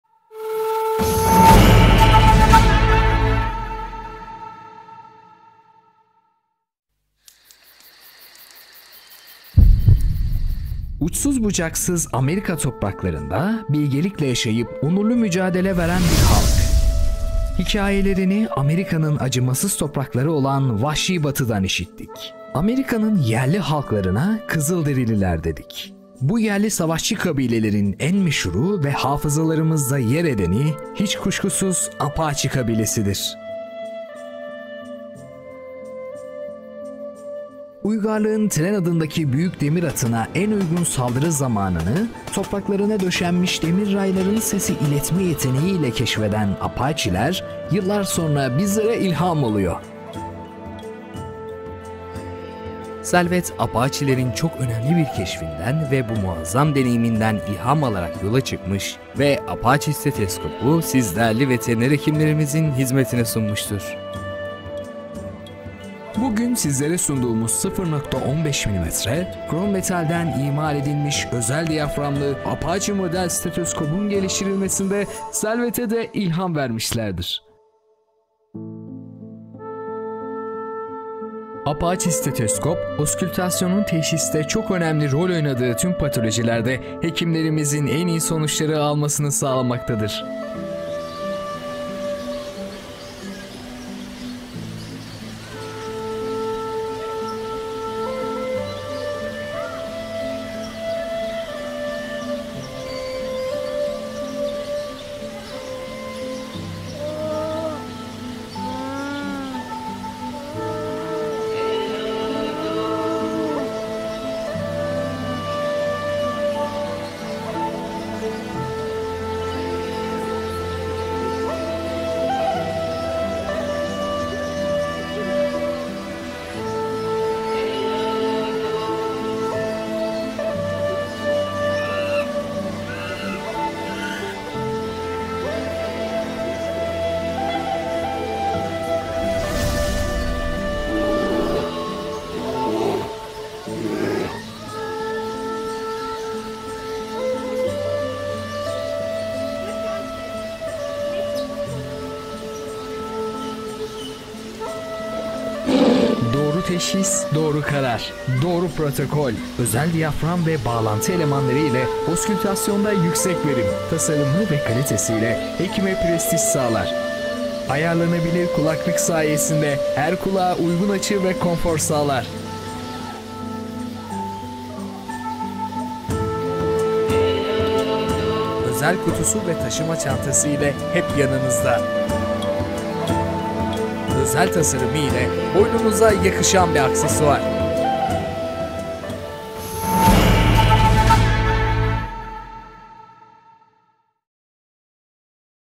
Veteriner Hekimler için Apache'nin Hikayesi | TR Tanıtım Videosu
Veteriner Hekimler için Özel Stetoskop
Uçsuz bucaksız Amerika topraklarında bilgelikle yaşayıp, onurlu mücadele veren bir halk;
Hikâyelerini Amerika’nın acımasız toprakları olan Vahşi Batıdan işittik. Amerika’nın yerli halklarına Kızılderililer dedik. Bu yerli savaşçı kabilelerin en meşhuru ve hafızalarımızda yer edeni hiç kuşkusuz Apache kabilesidir.

Uygarlığın tren adındaki büyük demir atına en uygun saldırı zamanını, topraklarına döşenmiş demir raylarının sesi iletme yeteneği ile keşfeden Apache’ler, yıllar sonra bizlere ilham oluyor.

Selvet Apache'lerin çok önemli bu keşfinden ve muazzam deneyiminden ilham alarak üretilmiş  Apache’yi siz değerli Veteriner Hekimlerimizin hizmetine sunmuştur.

Özel diyafram ve bağlantı elemanları ile oskültasyon da yüksek verim.

65 mm Krom Başlık
0.15 mm Krom Diyafram

Apache stetoskop oskültasyonun teşhisine çok önemli rol oynadığı tüm patolojilerde, hekimlerimizin en iyi sonuçları almasını sağlamaktadır.

Doğru teşhis!
Doğru karar!
Doğru protokol!